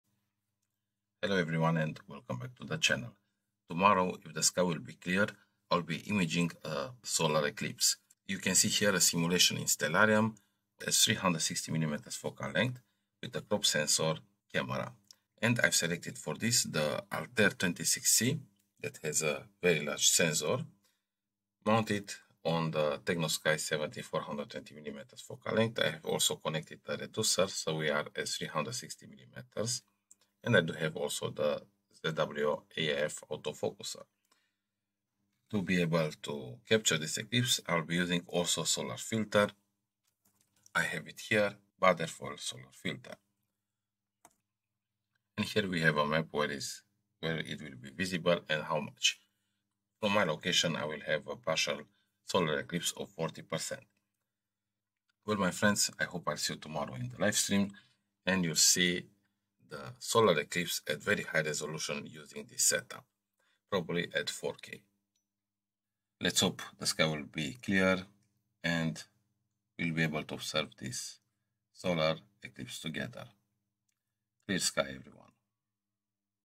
Solar Eclipse My Telescope & Camera Setup 25.10.2022
Getting ready to capture live the partial Solar Eclipse with very high resolution of my Altair 26c Hypercam, attached to my Tecnosky 70/420 mm telescope. Unfortunatly i have to cancel the live broadcast because of rain and clouds. I hope for better weather next solar eclipse